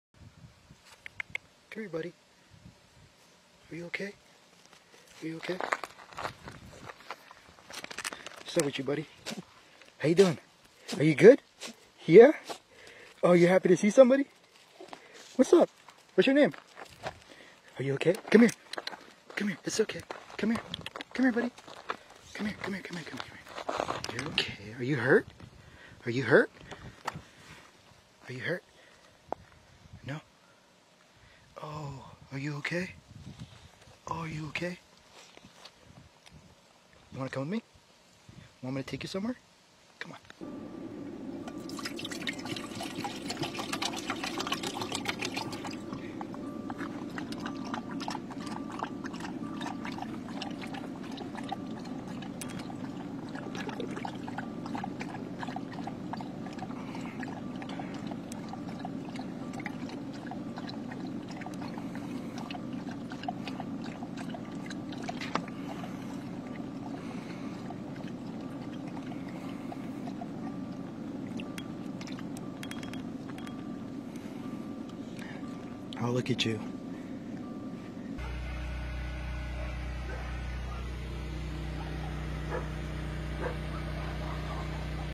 Puppy rescued from ruins of McKinney Fire
A photographer capturing the damage caused by California wildfires comes to the aid of a puppy wandering through the ruins of the area.